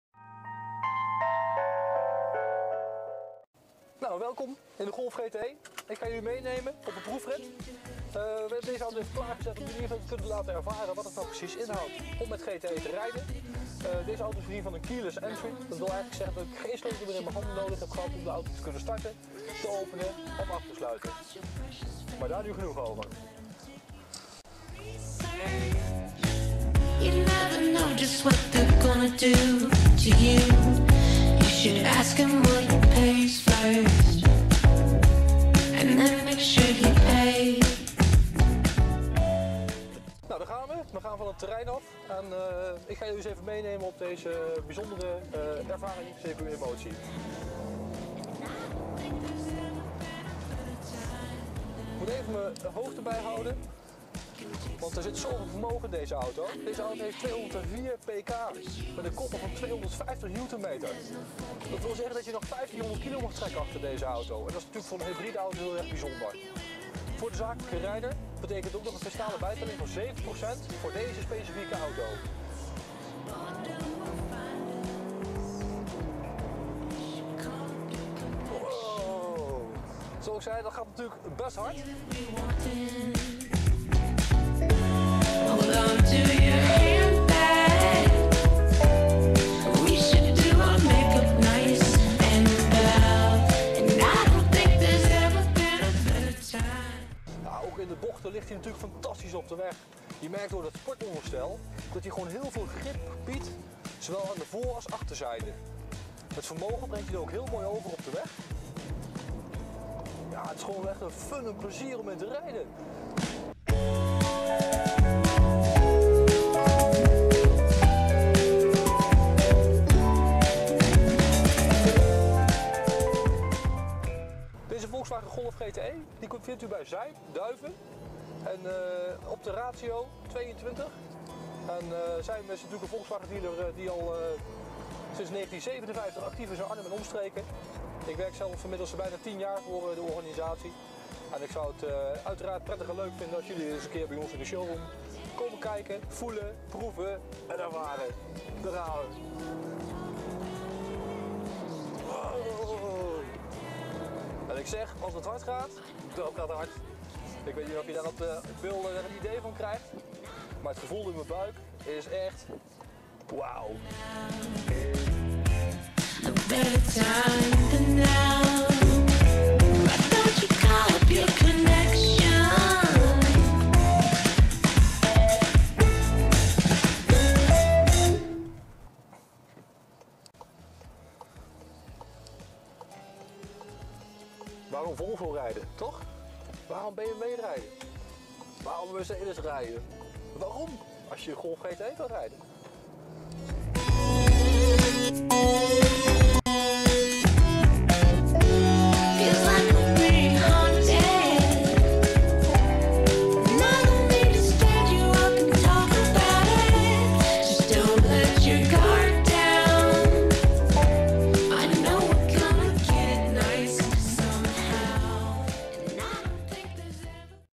Volkswagen Golf GTE 2016/ 2017 proefrit review test Zijm Duiven Jeroen Lans
Proefrit met de Volkswagen Golf GTE, Zijm Duiven, Jeroen Lans.
Golf GTE, Nogaro, Leder, Schuifdak, Xenon, Gaat hard!